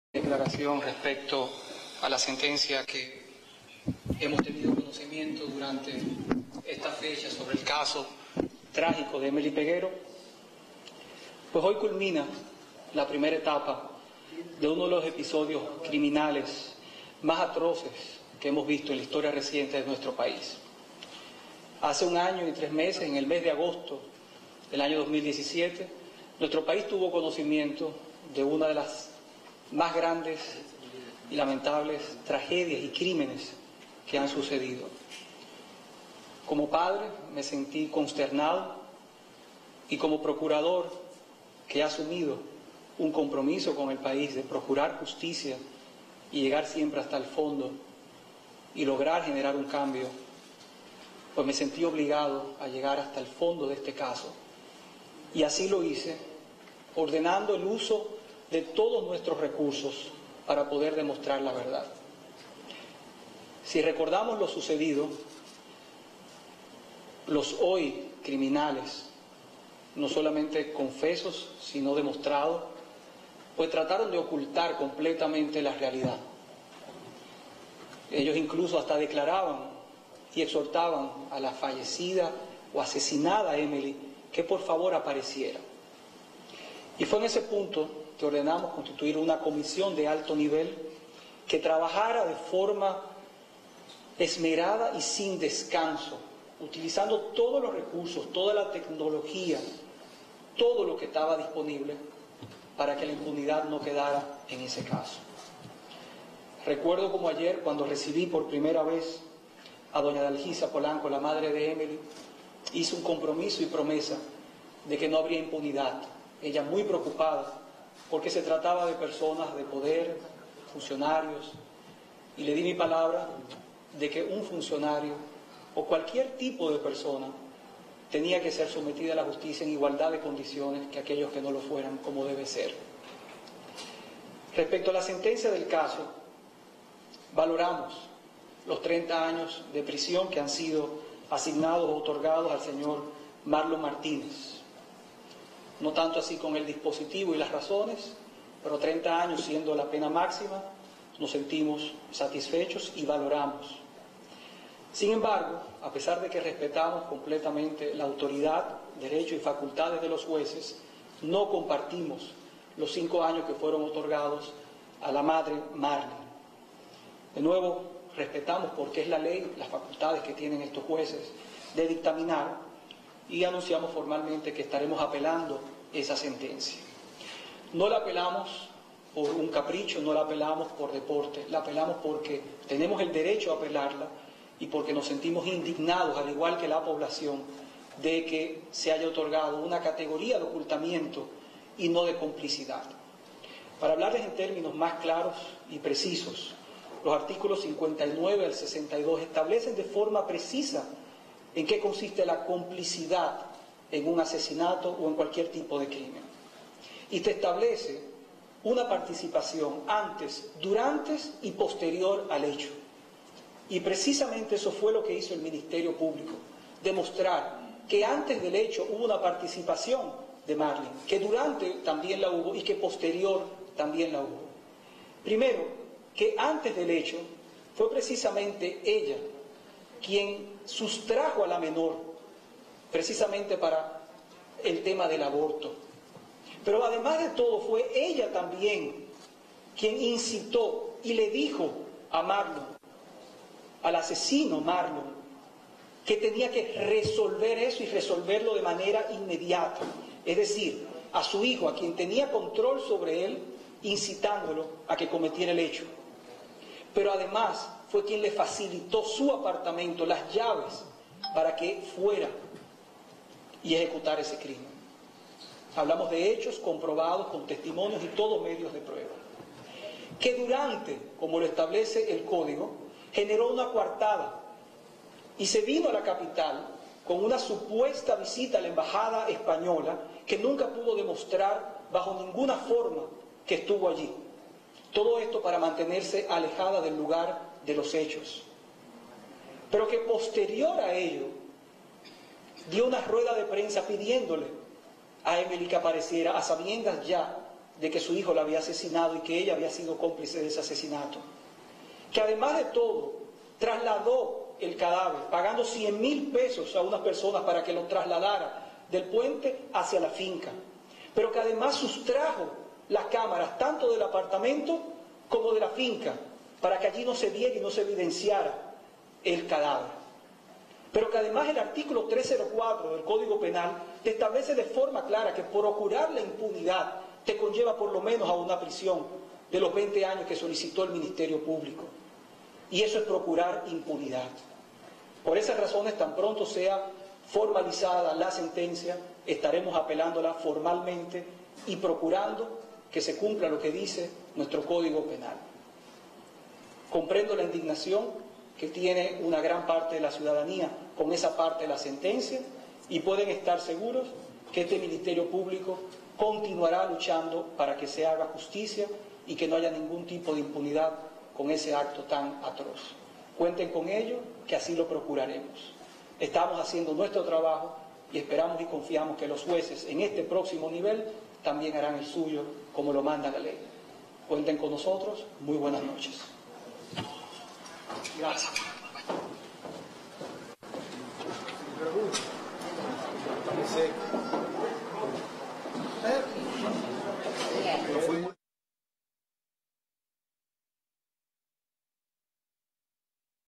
Procuraduría General apelará la sentencia que condenó a Marlin Martínez
Durante una rueda de prensa en la Procuraduría General de la República, el procurador Jean Rodríguez informó que el Ministerio Público apelará la sentencia que condenó a Marlin Martínez a cinco años de prisión por la muerte de la adolescente embarazada Emely Peguero.